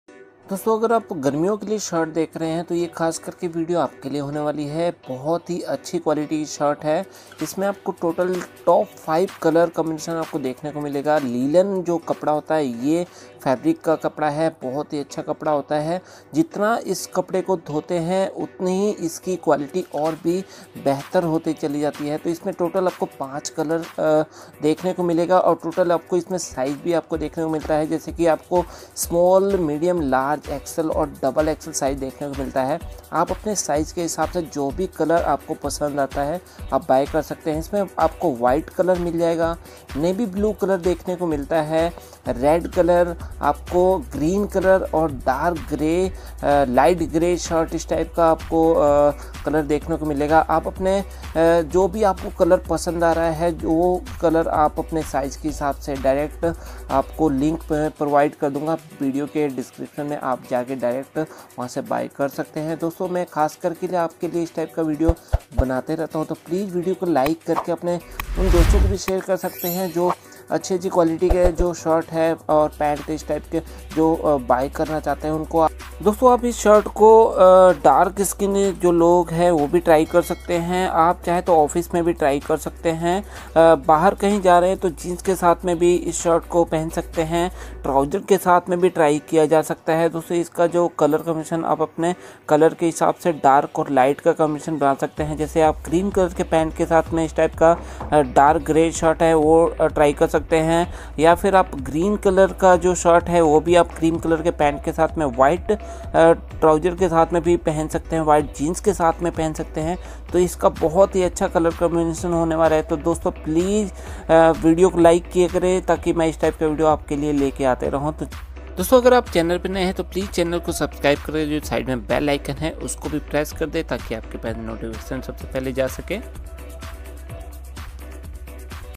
TOP 5 STYLISH SHIRTS 2023 | Office Shirt | Casual Shirt | Dark Men Fashion
Dosto ap is video me top 5 shirt dekhne wale hai jo ap office me pehn skte hai ya khi bahr ja rhe h to jeans ke sath pehn skte hai trouser ke sath me try kr skte hai linen fabric quality ki shirt hai bht achi quality hoti h dosto is shirt ko dark skin tone ke log bhi pehn skte hai